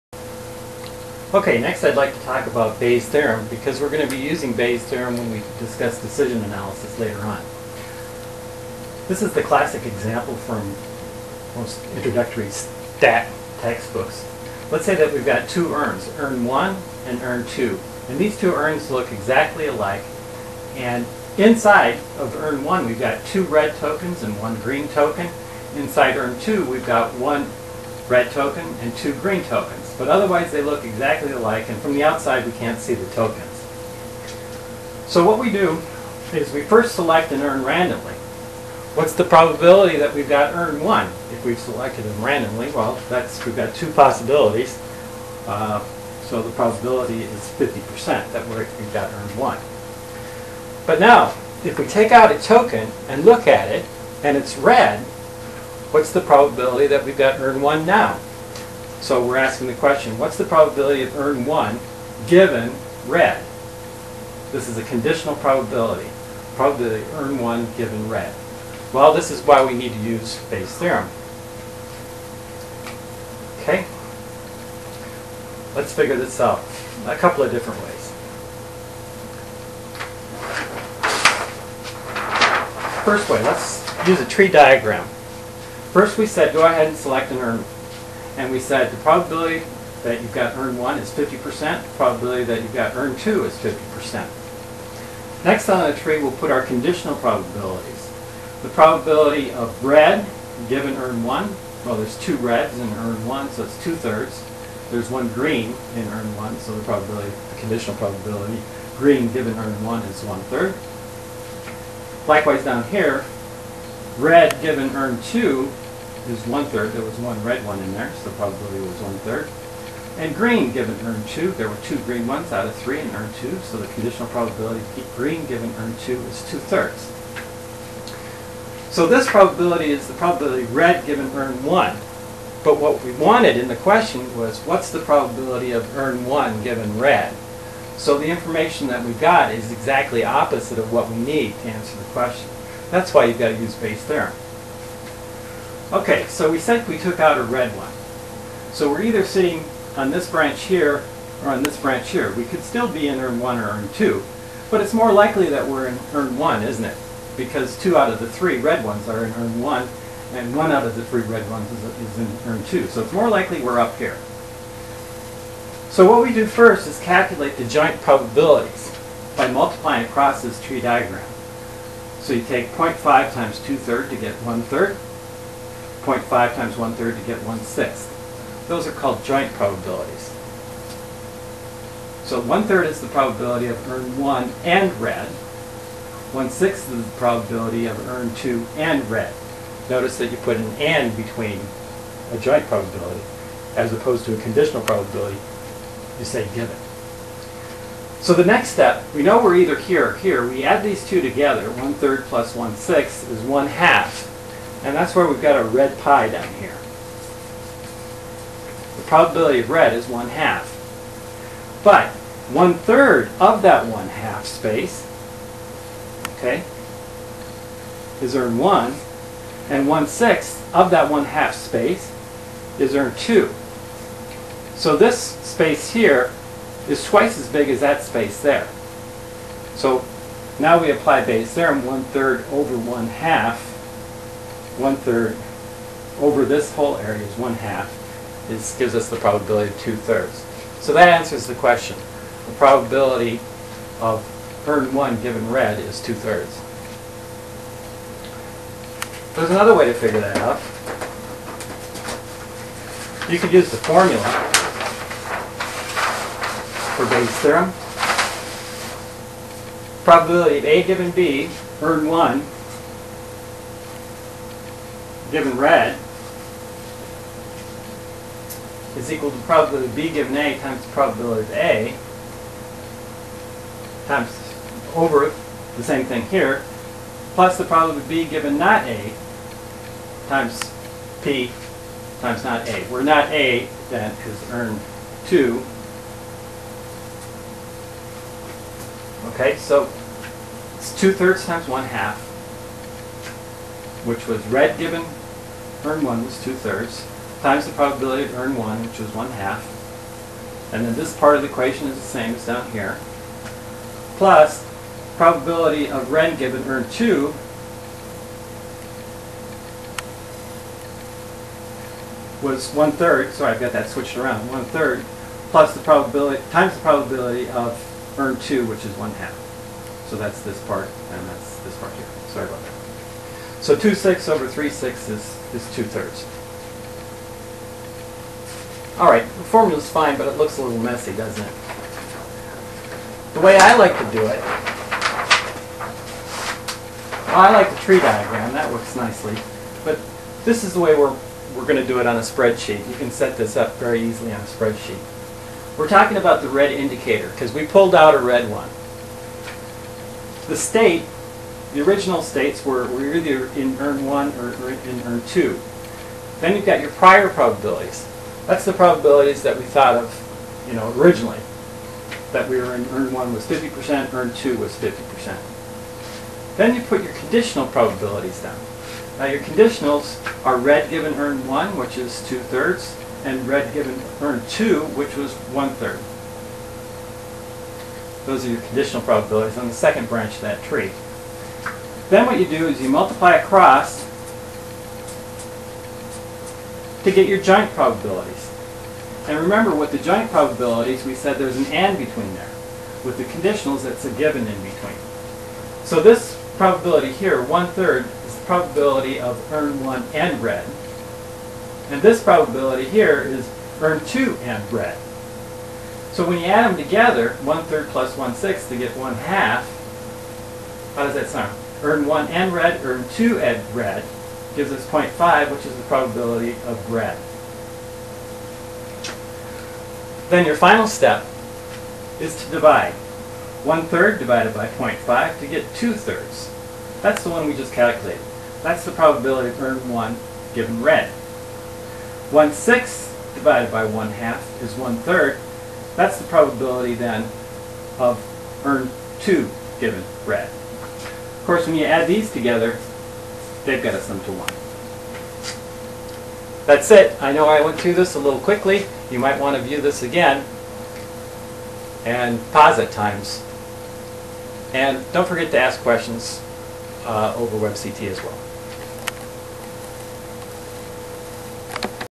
bayes theorem
We discuss the use of Bayes Theorem.  In decision analysis, we'll use it when the probabilities that we have are exactly opposite of what we need.  E.g. if we have P(A Given B) but we need P(B Given A) and we don't have all the joint probabilities, then Bayes applies.